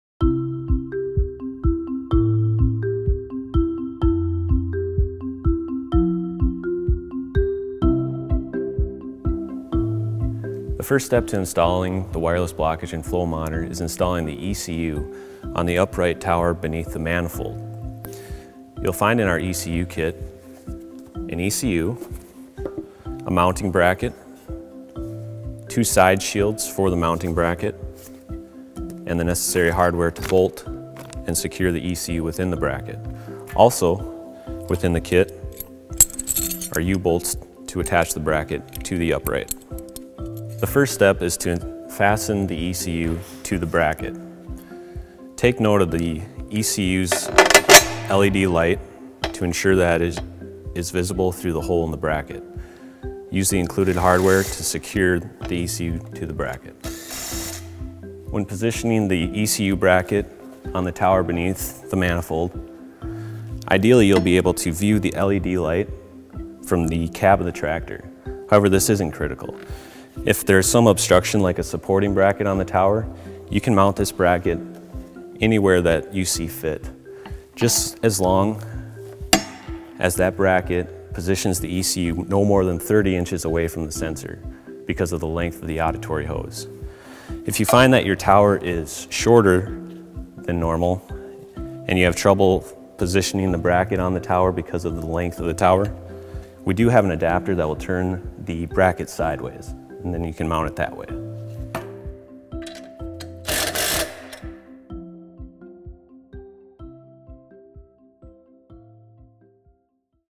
How to install the Recon Wireless Blockage and Flow Monitor: Part 2 - ECU
Part 2 of an 8-video series on the easy installation of the Recon Wireless Blockage and Flow Monitor. In this video, see how to install the ECU.